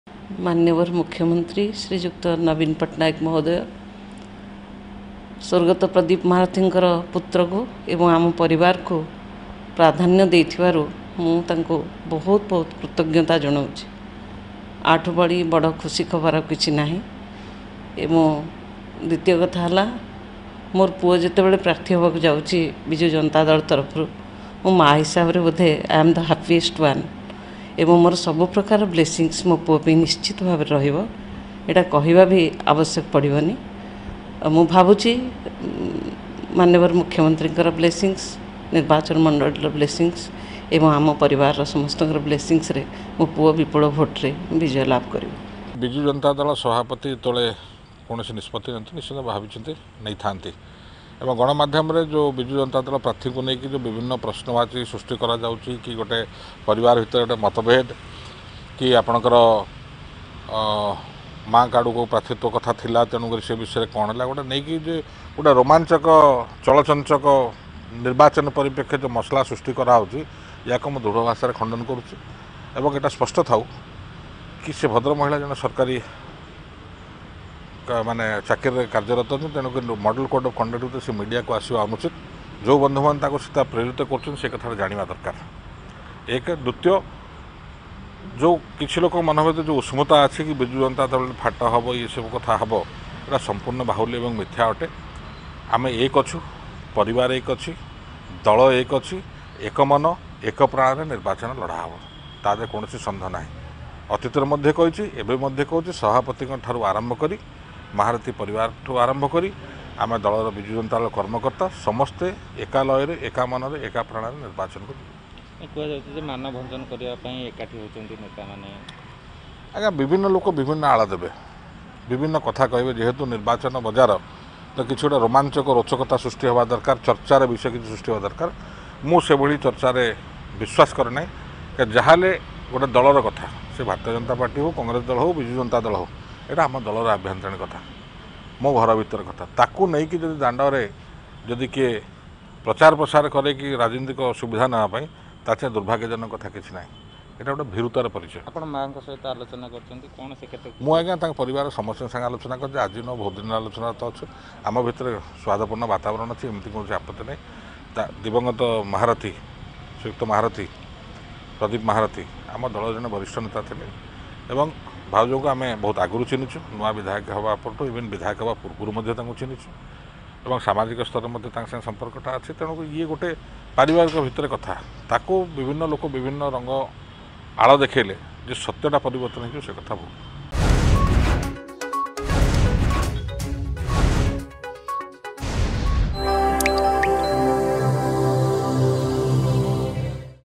ମୁଖ୍ୟମନ୍ତ୍ରୀ ଙ୍କୁ କୃତ୍ତଜ୍ଞତା ଜଣାଇଲେ ପ୍ରତିଭା ମହାରଥୀ...
ସ୍ବର୍ଗତ ପ୍ରଦୀପ ମହାରଥୀଙ୍କ ପରିବାର କୁ ପ୍ରାଧାନ୍ୟ ଦେଉଥ୍‌ବାରୁ ମୁଖ୍ୟମନ୍ତ୍ରୀ ଙ୍କୁ କୃତଜ୍ଞତା ଜଣାଇଲେ ପ୍ରତିଭା ମହାରଥୀ...